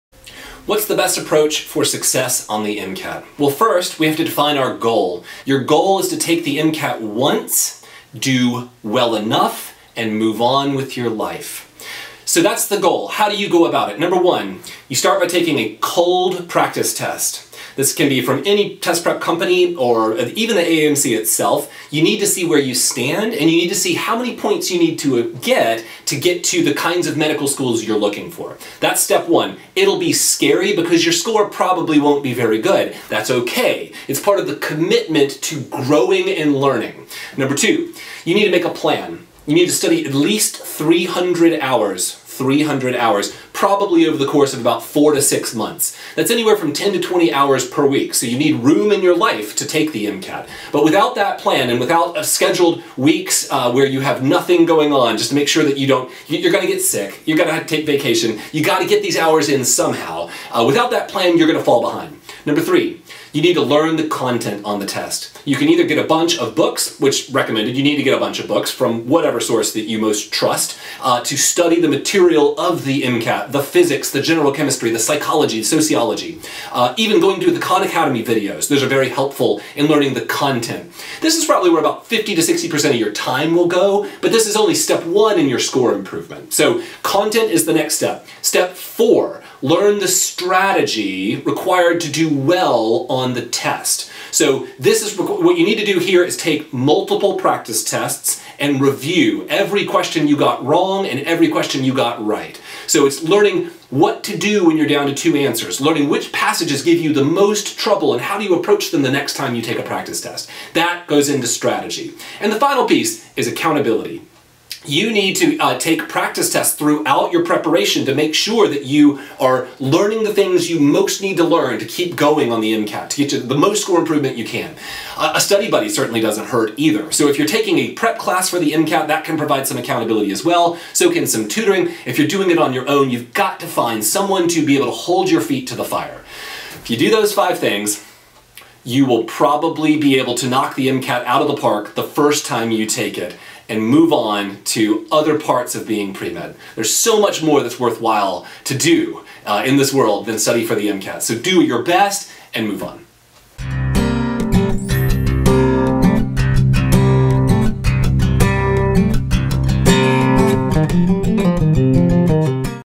5 Strategies to Knock Out the MCAT the First Time
Get the score you need on the MCAT, the first time you take it. Rob Humbracht, expert admissions advisor and former head instructor at the Princeton Review gives you 5 strategies to conquer the MCAT once and for all.